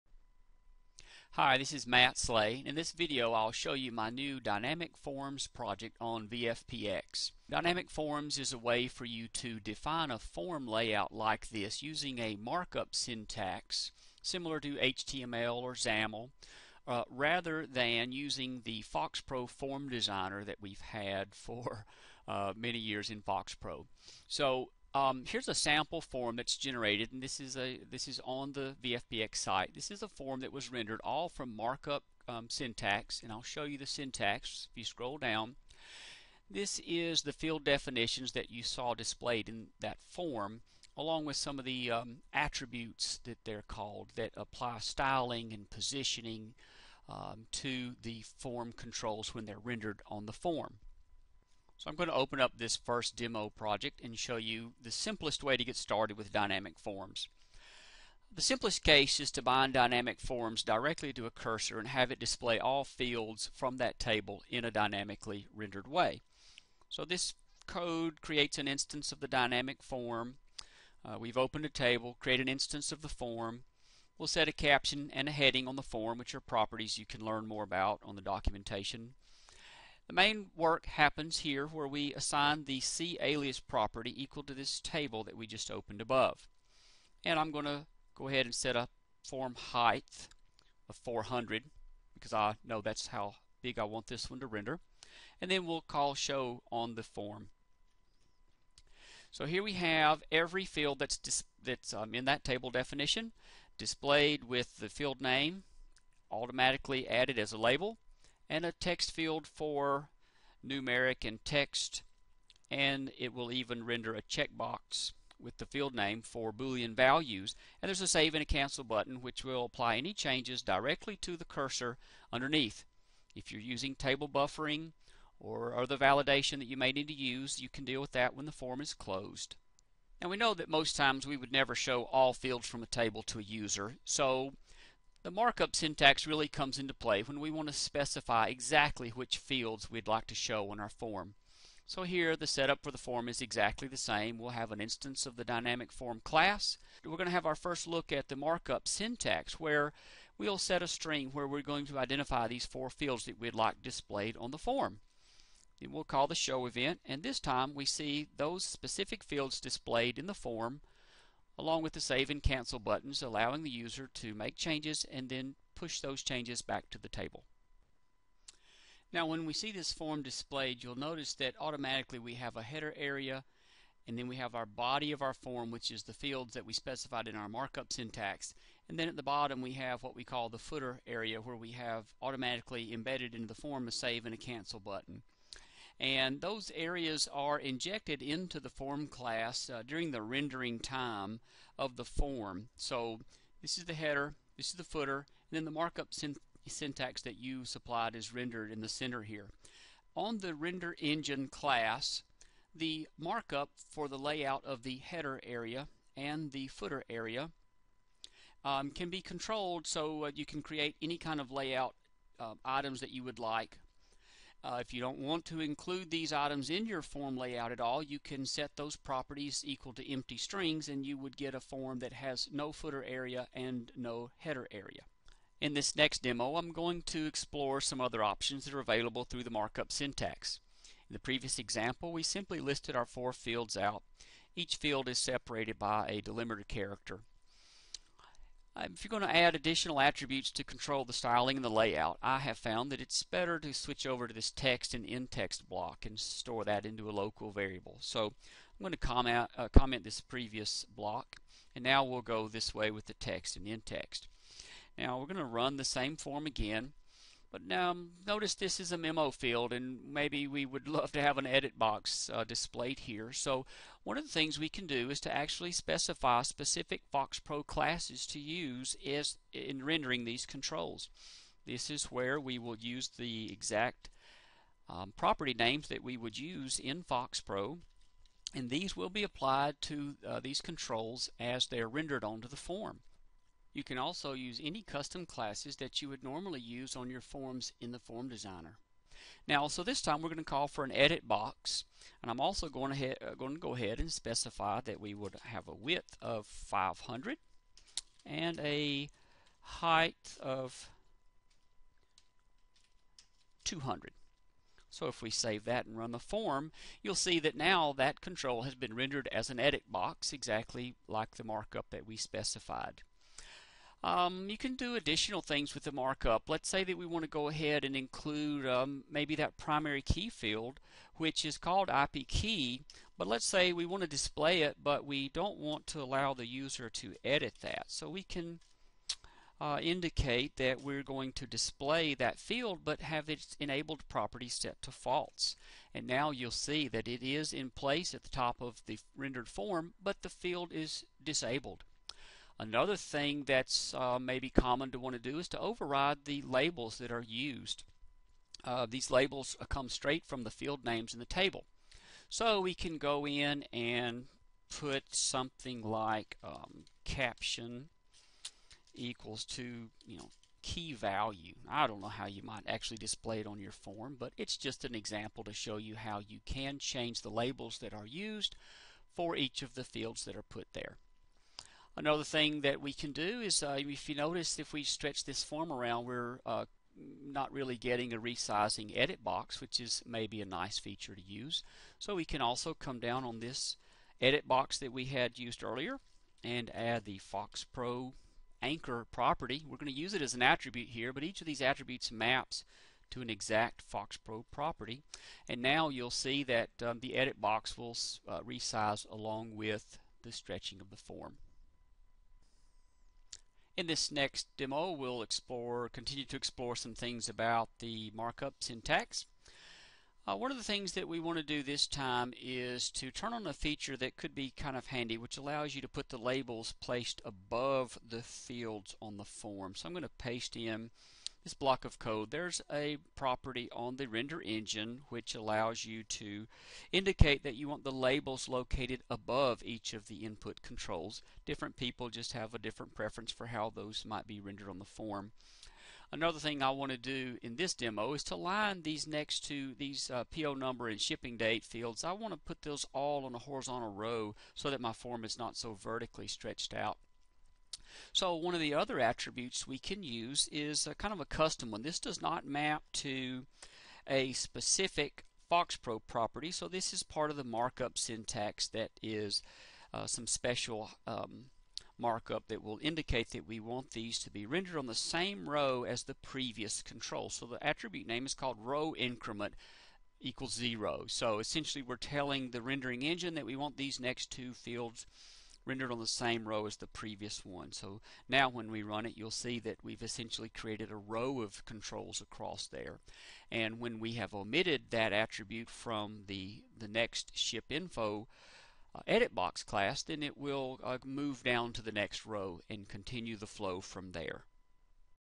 Visual FoxPro Dynamic Forms - Introduction
Dynamic Forms is a new way to create forms based on a -markup syntax- similar to HTML and XAML, in which you specify both the layout and ControlSources for the various controls to be displayed on the form.

The result will be a dynamically constructed form, specified in code, rather than manually creating it in the Form Designer.

This video introduces Dynamic Forms with 4 code sample walk-throughs to generate various types of Dynamic Forms.